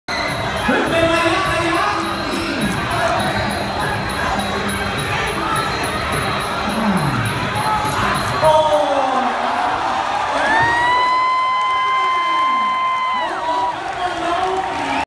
Katya Knysheva 7-0-0 / Bangla Boxing Champion / Muay Thai, Phuket, Thailand
Kata Phuket Top Team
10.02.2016
Phuket, Thailand